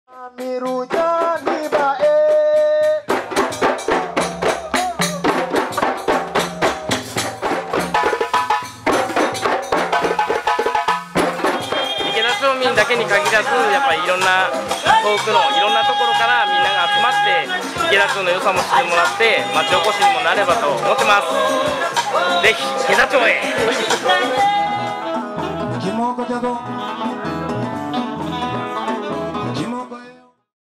アフリカ文化で町おこし 「気質が似ている」 岐阜県池田町
アフリカの音楽や文化で町おこしを目指す集まりが、濃尾平野の北端にある池田町にある。グループ名は「池田アフリカン化計画実行委員会」。山地が３分の１を占める中山間地で、なぜ「アフリカ」なのか。